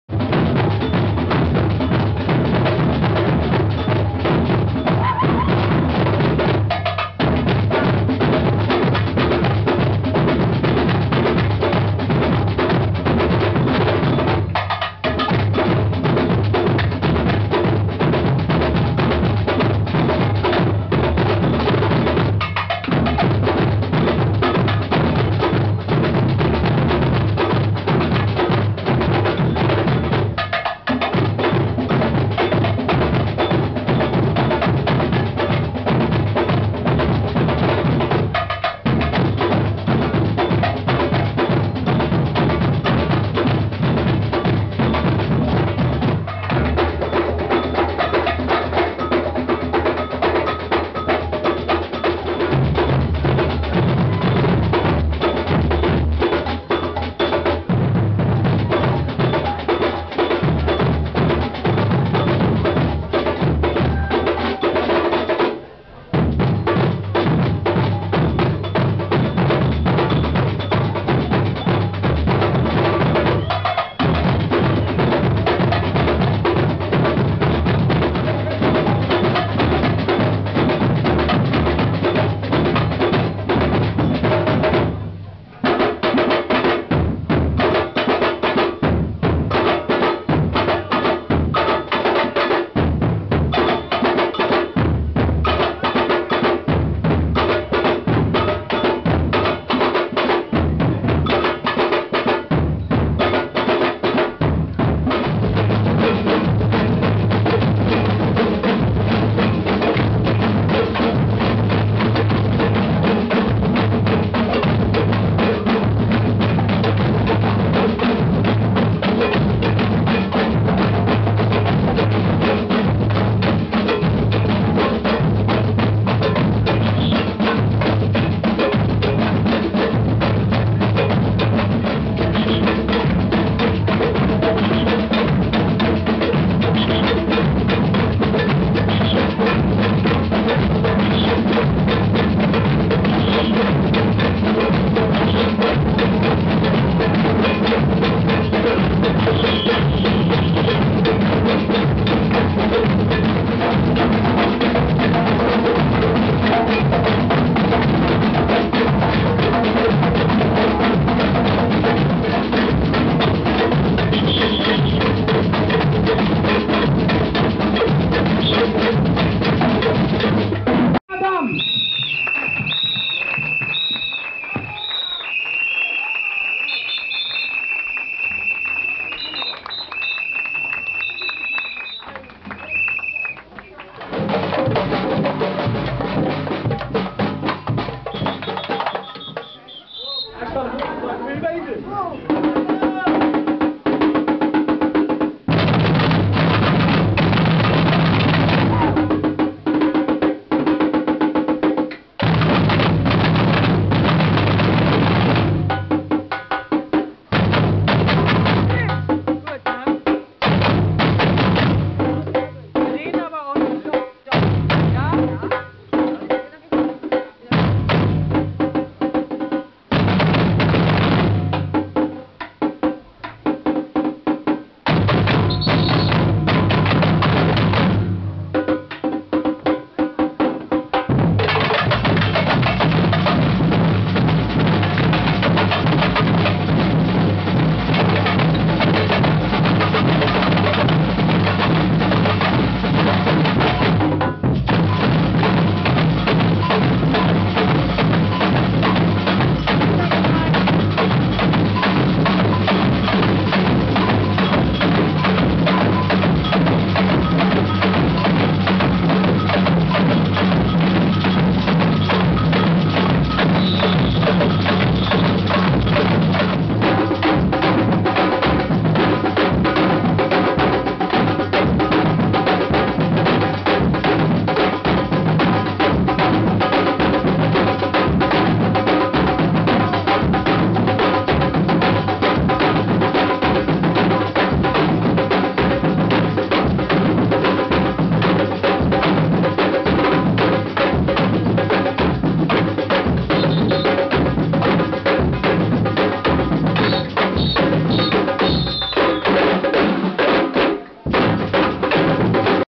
einheizen_samba
... samba karneval in bremen im lagerhaus und in der lila eule